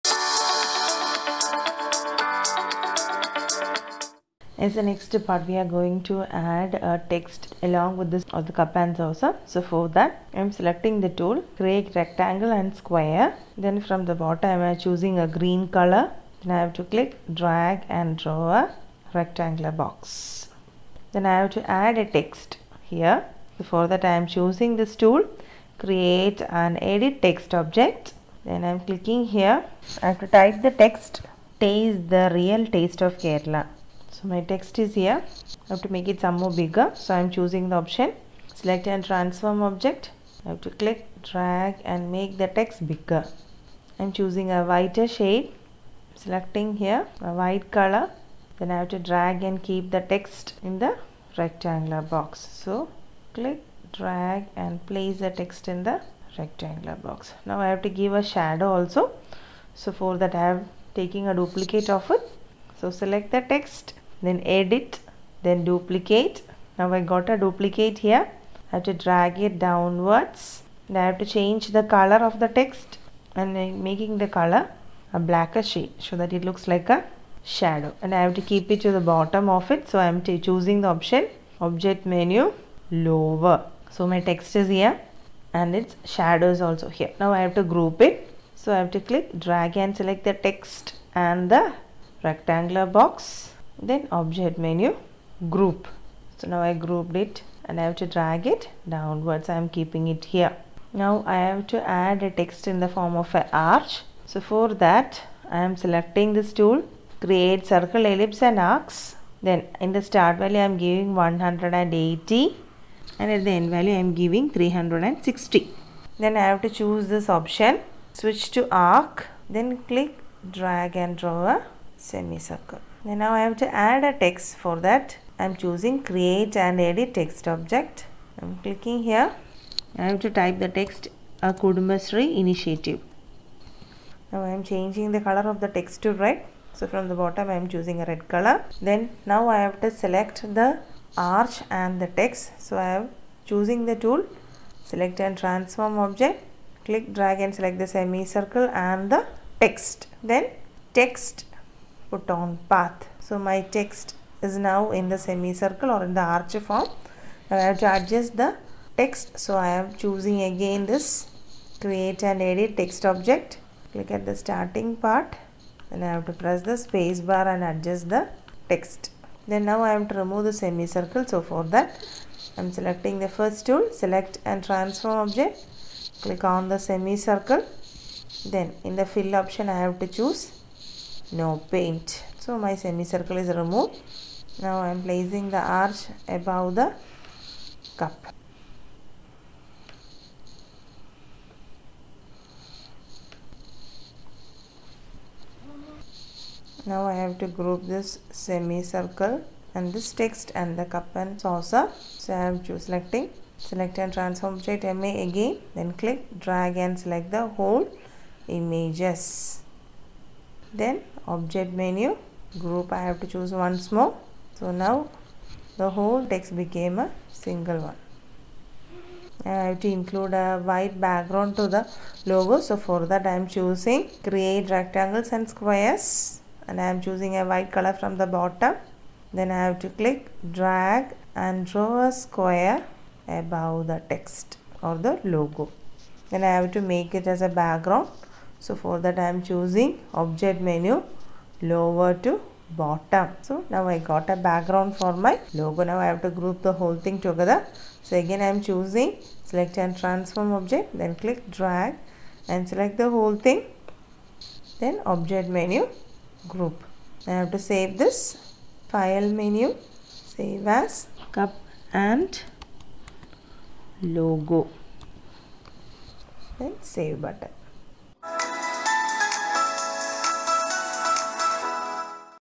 IT | Std 10 | Chapter 1 | Inkscape | Part 2 | Text in Arch Shape & Logo
Creating a Logo in Inkscape & a text created in the form of arch.